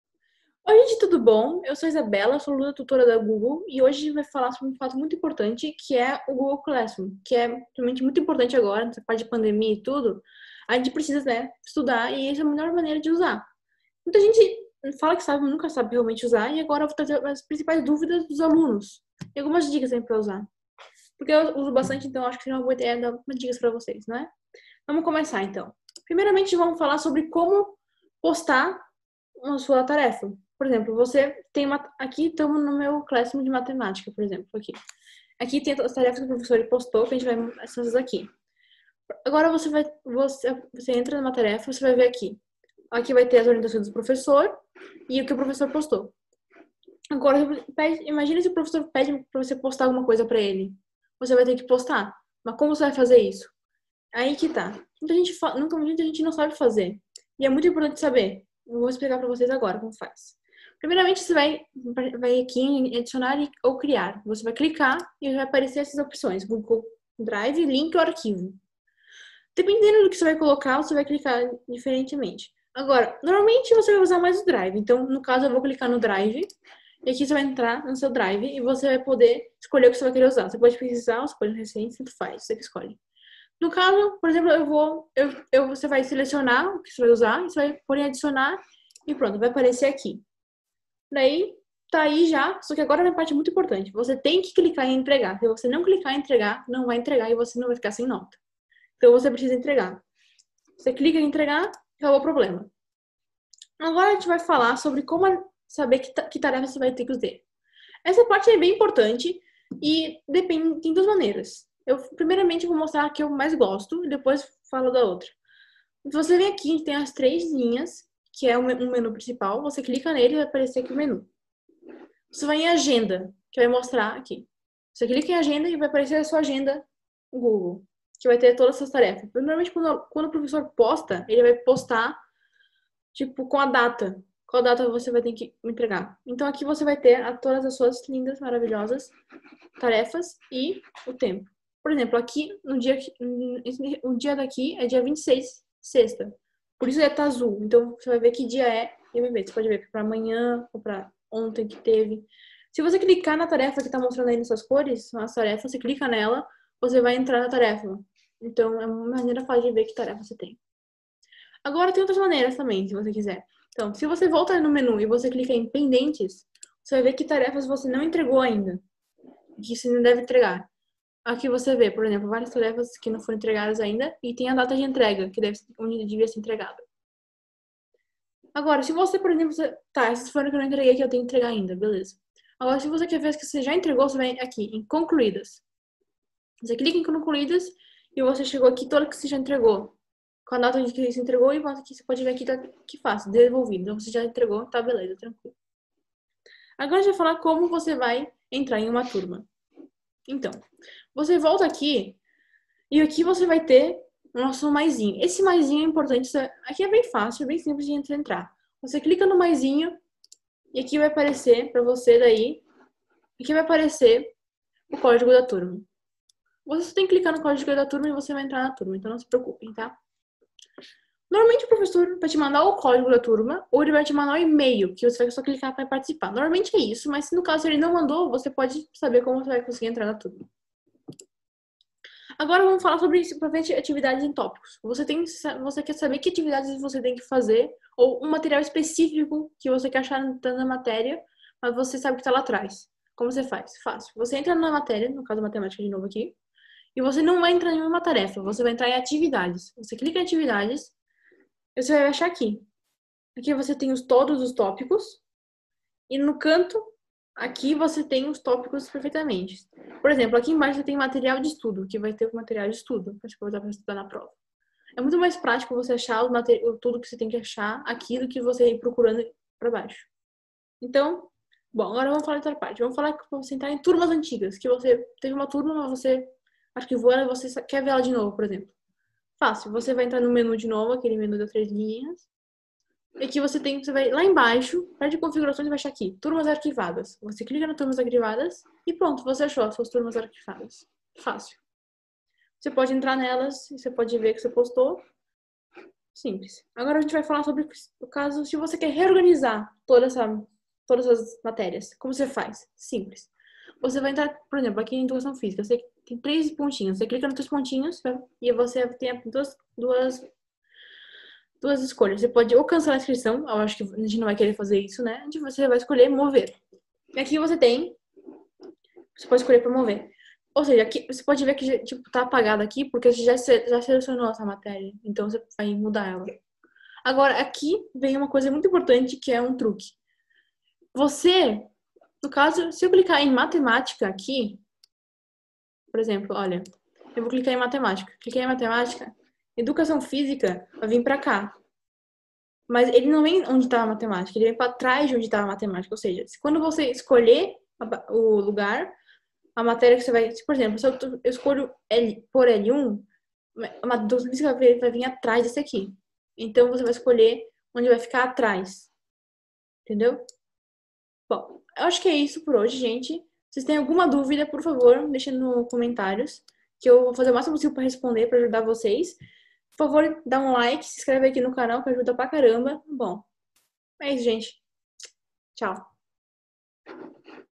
Dicas sobre o Google Classroom
Vamos entender como se usar essa feramente muito útil e importante! Contem comigo se tiverem qualquer dúvida!